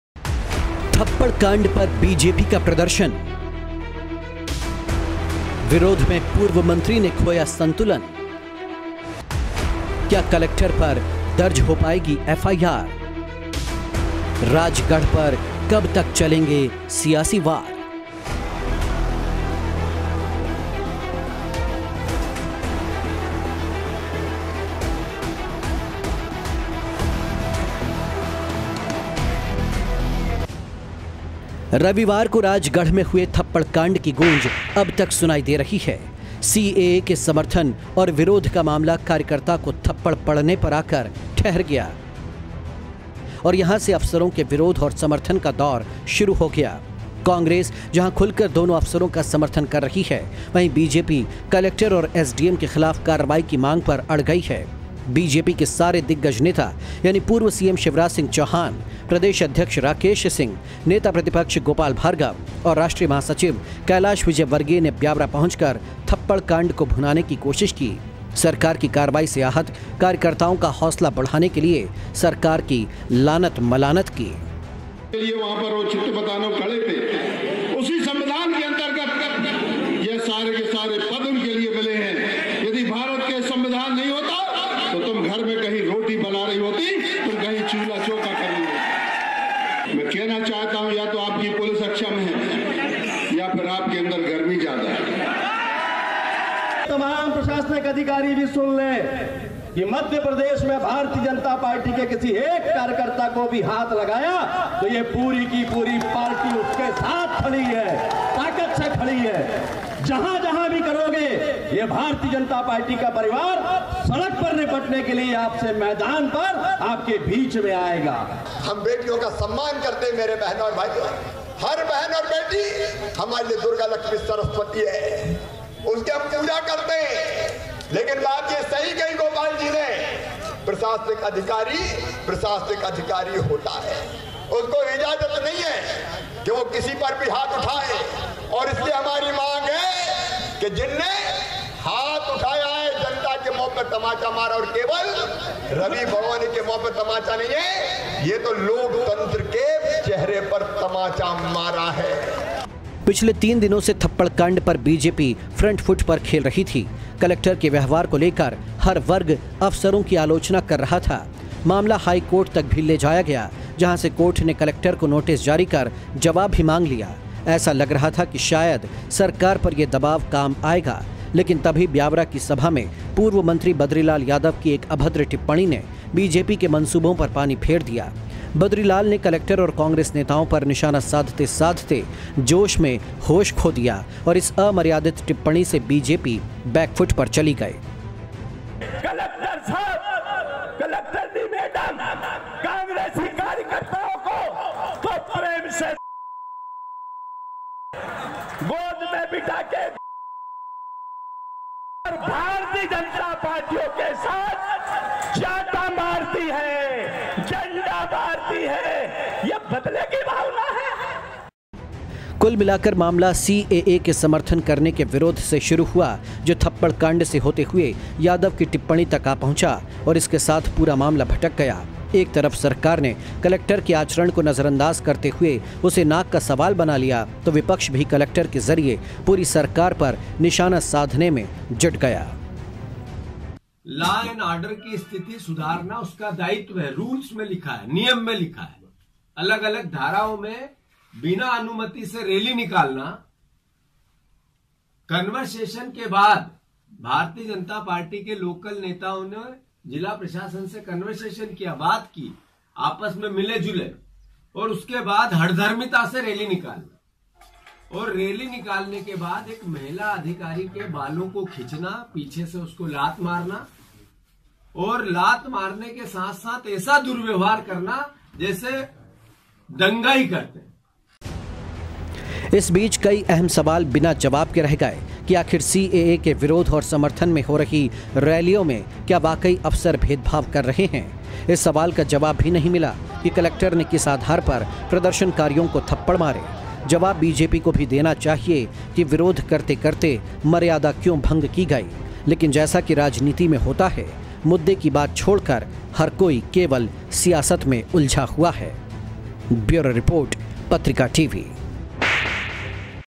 RAJGARH : थप्पड़ कांड पर बीजेपी का प्रदर्शन
#'RajgarhपरकबतकचलेंगेPoliticalWar?

रविवार को राजगढ़ में हुए थप्पड़ कांड की गूंज अब तक सुनाई दे रही है...सीएए के समर्थन और विरोध का मामला कार्यकर्ता को थप्पड़ पड़ने पर आकर ठहर गया और यहां से अफसरों के विरोध और समर्थन का दौर शुरु हो गया...कांग्रेस जहां खुलकर दोनों अफसरों का समर्थन कर रही है वहीं बीजेपी कलेक्टर और एसडीएम के खिलाफ कार्रवाई की मांग पर अड़ गई है...बीजेपी के सारे दिग्गज नेता यानि पूर्व सीएम शिवराज सिंह चौहान, प्रदेश अध्यक्ष राकेश सिंह, नेता प्रतिपक्ष गोपाल भार्गव और राष्ट्रीय महासचिव कैलाश विजयवर्गीय ने ब्यावरा पहुंचकर थप्पड़ कांड को भुनाने की कोशिश की...सरकार की कार्रवाई से आहत कार्यकर्ताओं का हौसला बढ़ाने के लिए सरकार की लानत-मलानत की...